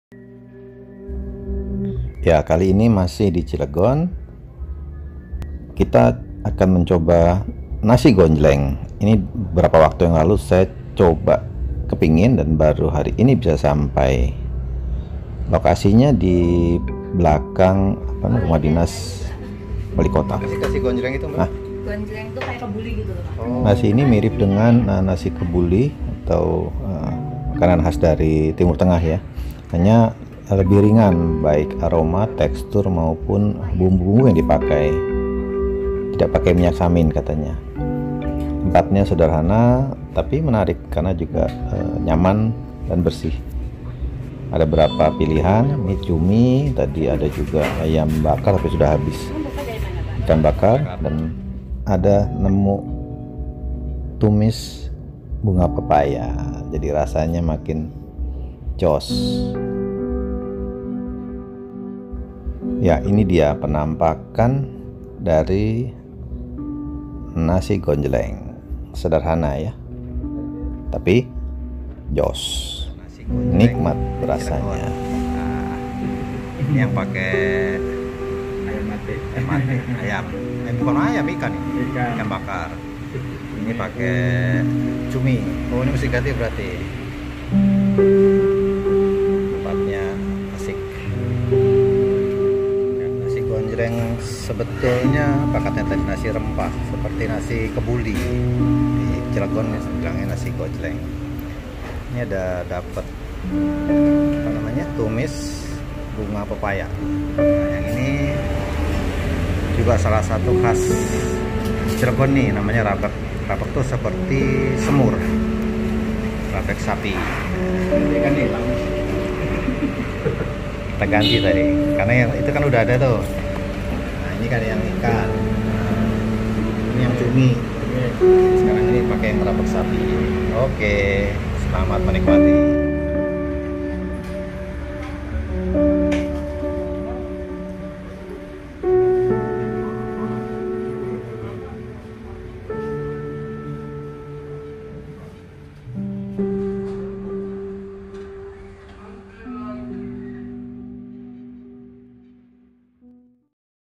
nasi GONJLENG khas Cilegon
Nasi gonjleng merupakan salah satu kuliner olahan nasi khas yang banyak ditemukan.. spt tradisi menu masakan tersebut. Bentuk dan rasanya mirip dan menyerupai nasi samin atau nasi kebuli khas Timur Tengah hanya lebih ringan.
kombinasi dg menu lokal spt, ikan/ayam bakar, tumis cumi juga bunga pepaya

hmm .. yumm yum